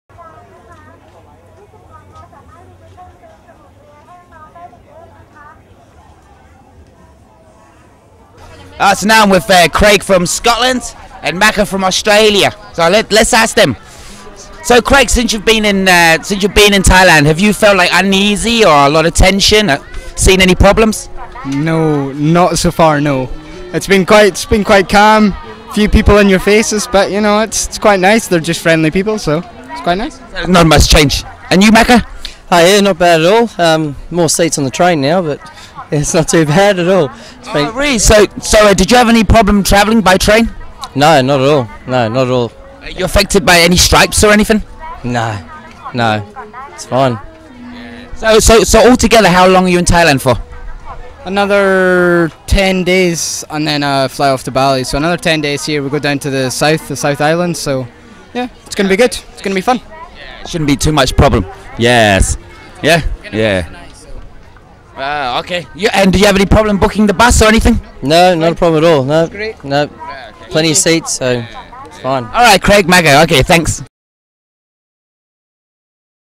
Thailand Tourism Situation (Chatuchak Market - Eng 4)
The interview is taken on 7th September 08 from a foreign tourists visiting Chatuchak Market regarding the political situation. Upon interview, tourists state the situation has neither affected their visit to Thailand nor to the tourism in Thailand. The are not affected by any transportation problem and any strike. Perhaps, they feel safe and calm on the "Land of Smiles".